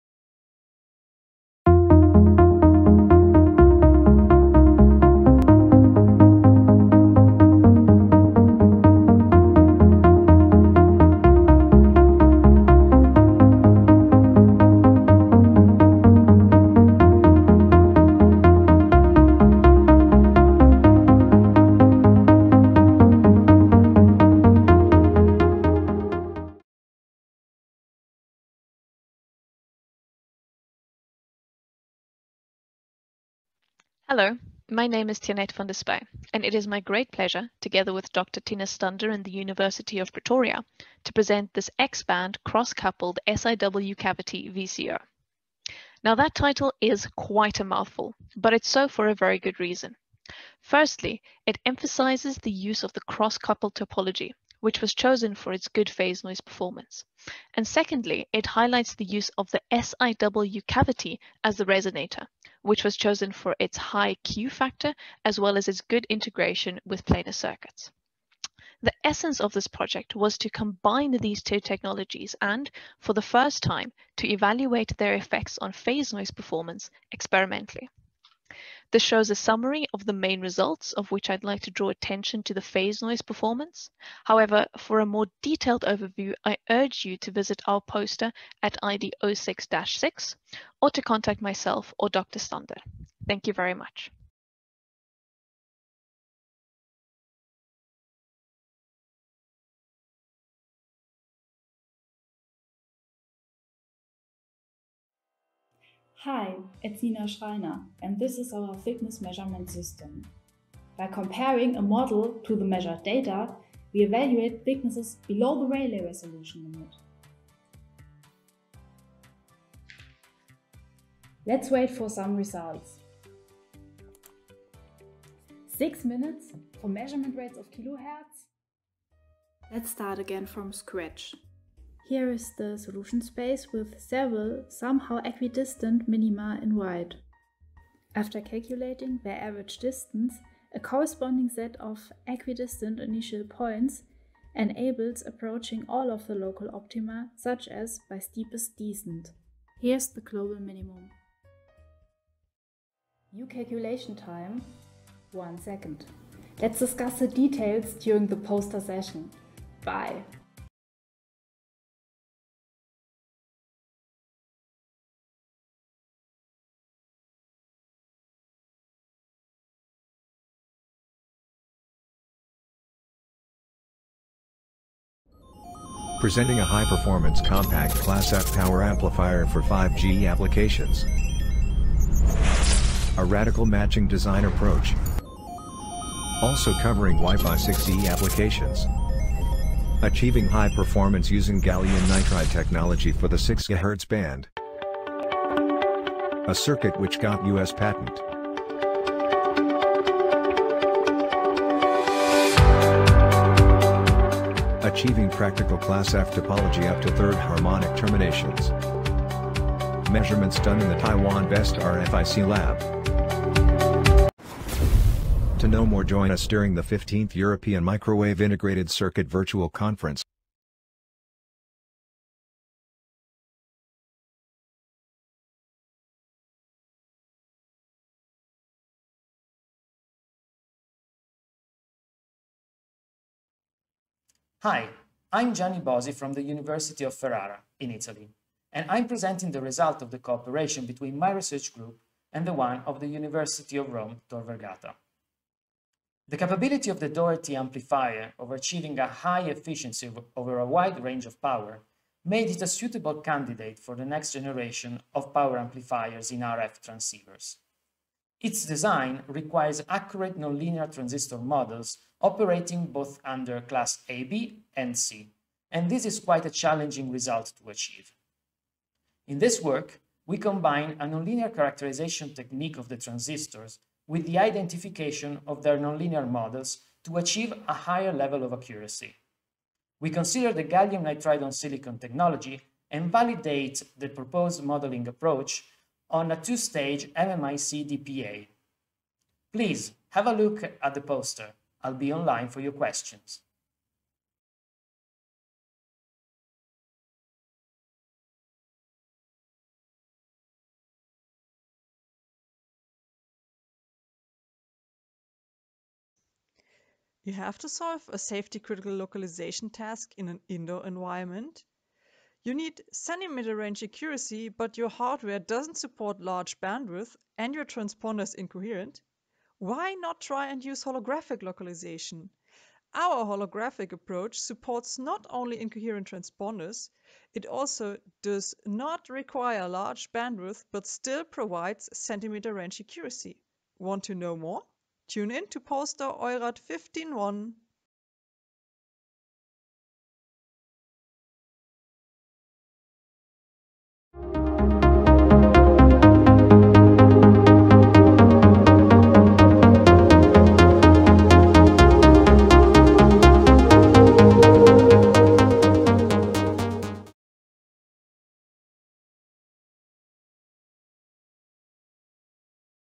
EuMW2020 Poster pitch winners
5 best-pitch prize winners of EuMW2020:

* An X-band Cross-Coupled SIW Cavity VCO, T. van der Spuy, T. Stander

* Optimization Algorithms for Accurate FMCW Millimeter-Wave and Terahertz Thickness Measurements, N. Schreiner, M. Bortz, W. Sauer-Greff, R, Urbansky, F. Friederich

* A High-Efficiency Compact Class F GaN MMIC Power Amplifier for 5G Applications, R. Joshi, M.-H. Liu, S.S.H. Hsu

* Empowering GaN-Si HEMT Nonlinear Modelling for Doherty Power Amplifier Design, . Bosi, A/ Raffo. R. Giofre, V. Vadata

* On the Needlessness of Signal Bandwidth for Precise Holographic Wireless Localization, M. Lipka, S. Bruckner, E. Sippel, M. Vossiek